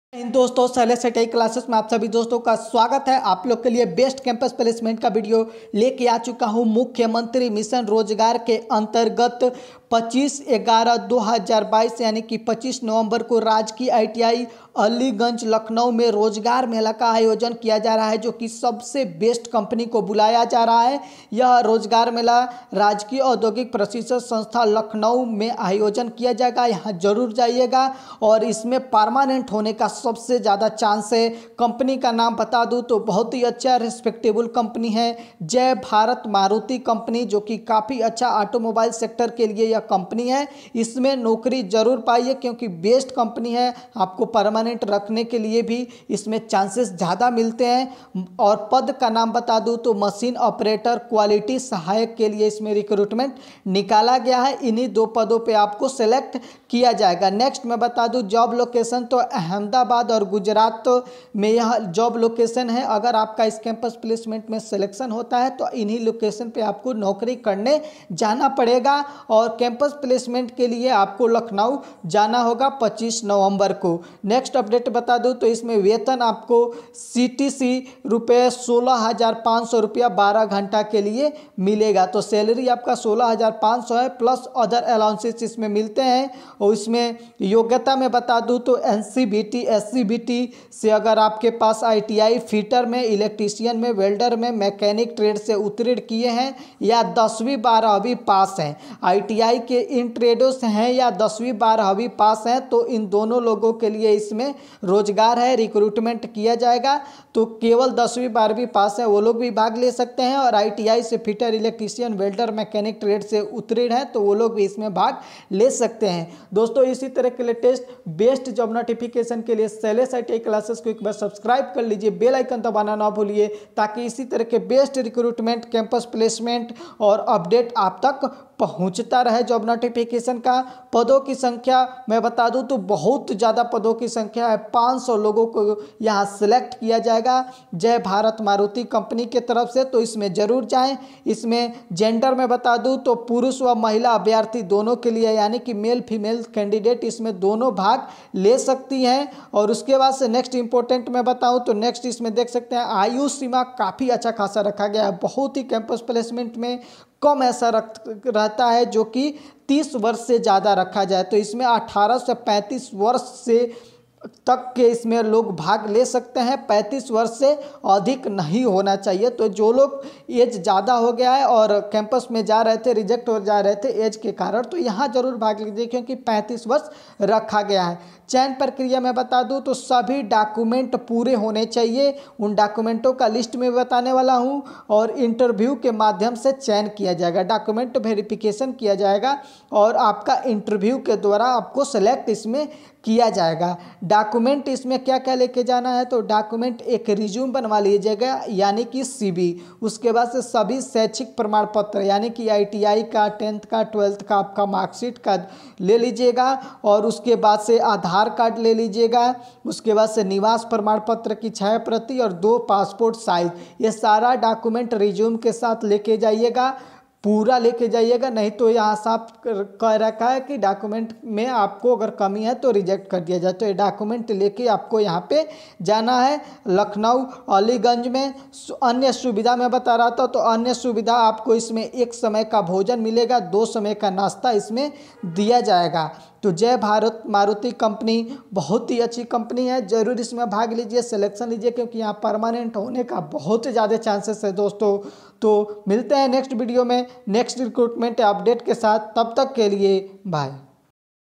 जय भारत मारूति कम्पनी में भर्ती || 10th+12th पास और ITI पास के लिए सुनहरा अवसर | Salary 16982 रु CTS
किसी भी डाउट को को पूछने के लिए या Direct company joining के लिए आप telegram group को join kar lijiye